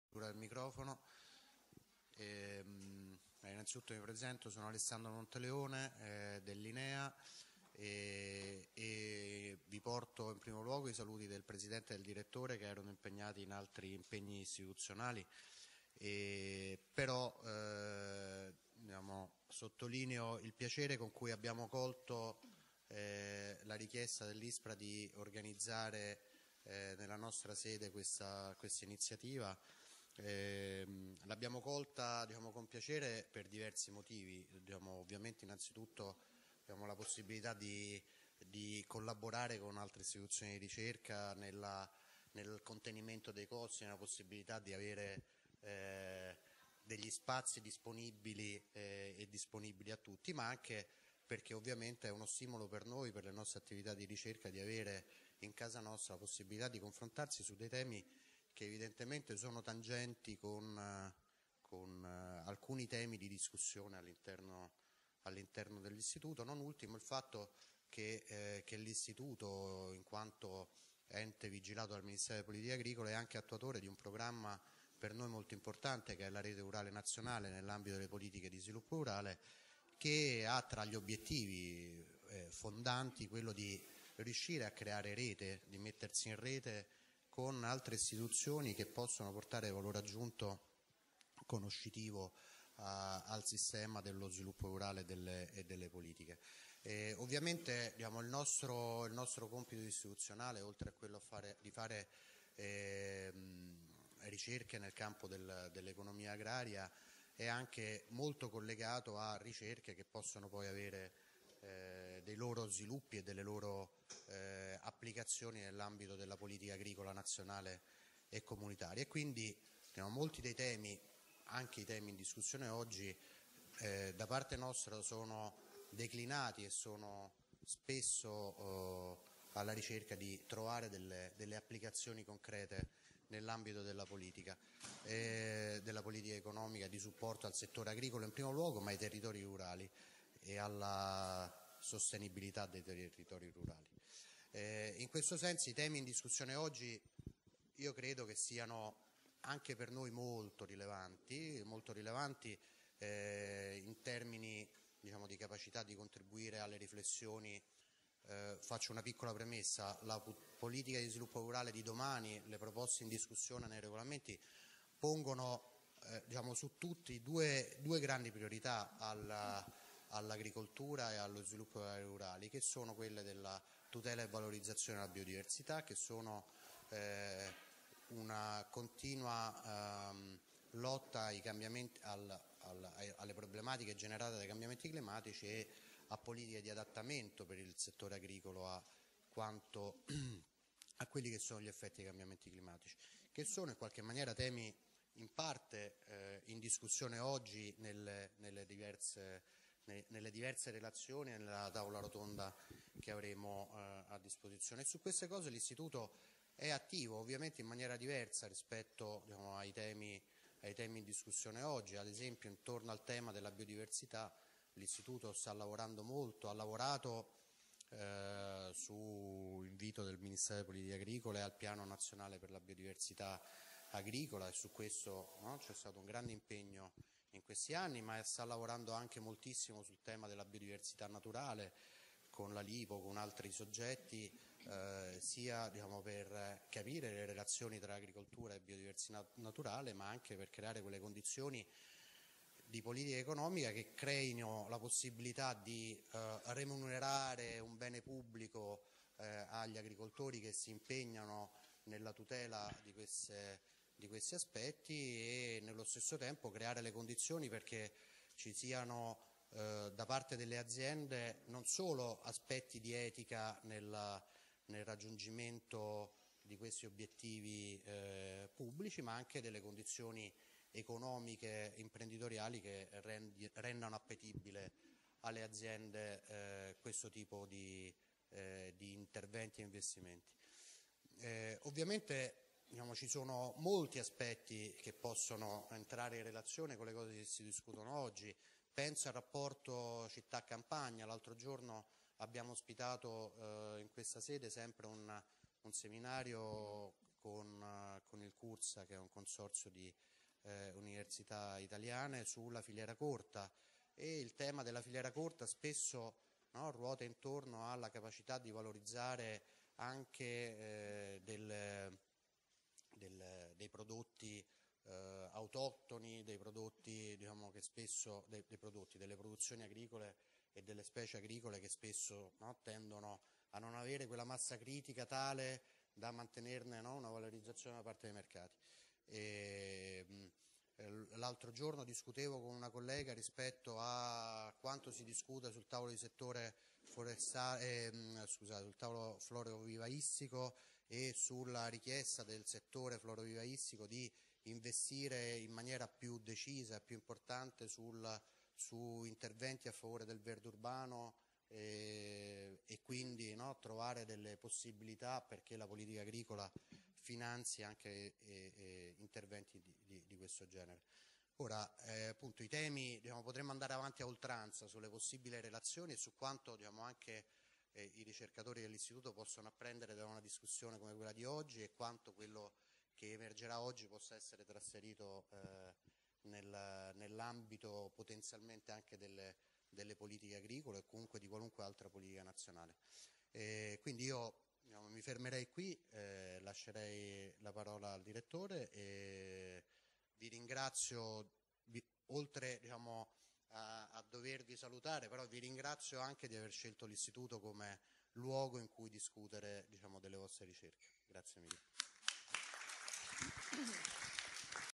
Monteleone (INEA)
Negli arredi urbani e peri-urbani di aree mediterranee solo le piante che vi vegetano spontaneamente possono contribuire concretamente al risparmio d'acqua ed alla riduzione del costo delle cure colturali. L'impiego di piante erbacee spontanee autoctone (fiori di campo o, all'inglese, wildflowers) negli spazi a verde è, però, una tecnica recente nel nostro paese ed è perciò necessaria l'intensificazione di studi al riguardo, sia per quanto riguarda la valutazione del comportamento in opera delle numerose specie della nostra flora, sia per il raggiungimento della conoscenza approfondita delle esigenze eco-fisiologiche delle piante più idonee.